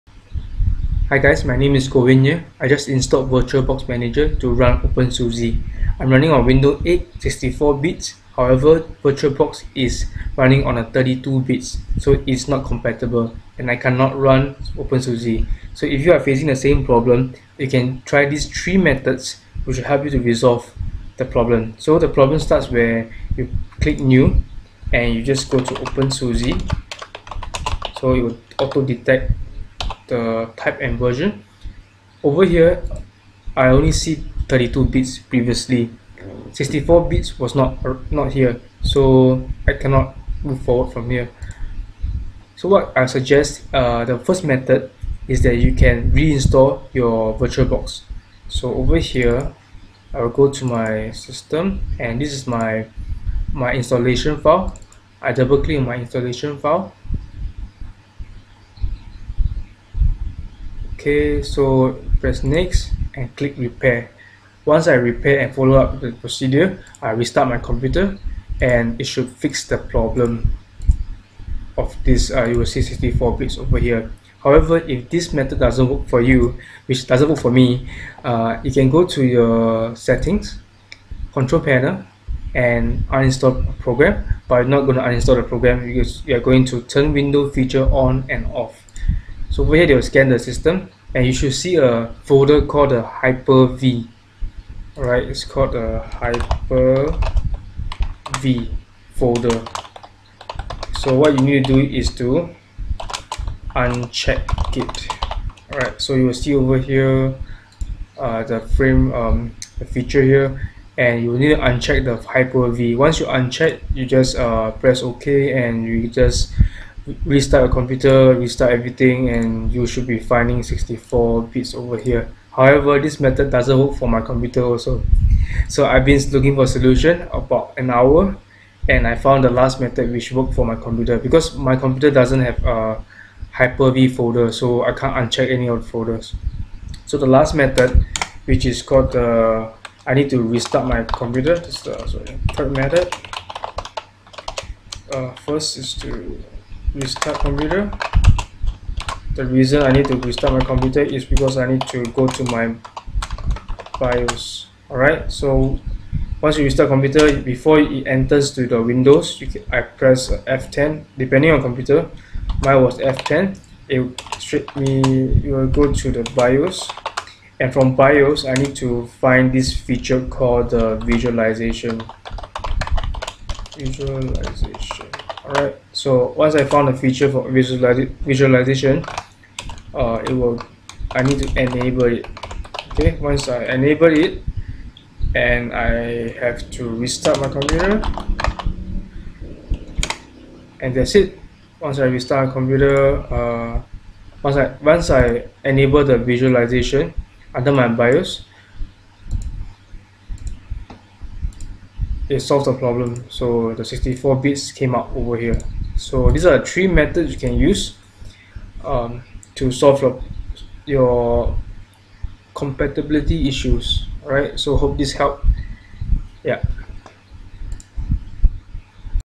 64 Bits Error VirtualBox 5.0.20 (3 Solutions)
If you are having the same problem I had, you can try these 3 methods. I'm running on Windows 8 (64 bits). You may want to share with your classmates.

Also, join me & check out the link below on 12 Foundations: What They Should've Taught Us In School About Health, Wealth, Love & Happiness!